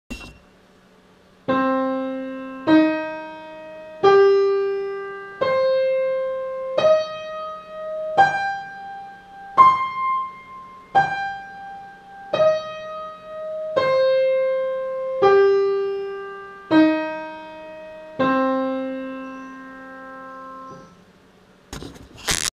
C minor arpeggio on piano 2 octaves (slow with right hand)
Fingers: 123 1235 (back down is the same but backwards)

The only 3 notes that appear in the C minor arpeggio are C, Eb, and G.  If you are playing any other note it's a wrong one!